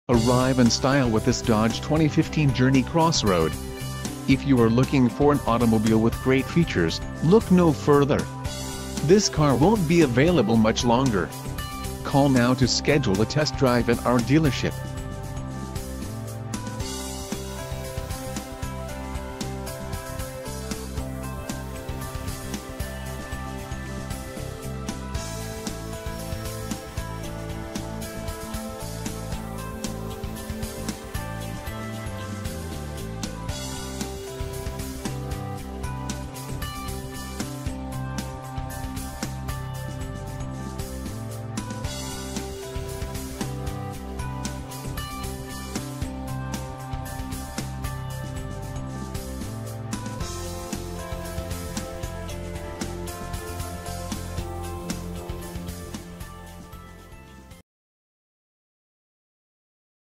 2015 Dodge Journey Crossroad - Roper Honda - Joplin, MO 6...
For Sale in Joplin, MO 64801

Used 2015 Dodge Journey Crossroad, white exterior, black interior, 6, 3.6l, automatic, all-wheel drive, 4 doors, 106,338 miles, Stock H613281, VIN: 3C4PDDGG6FT684674,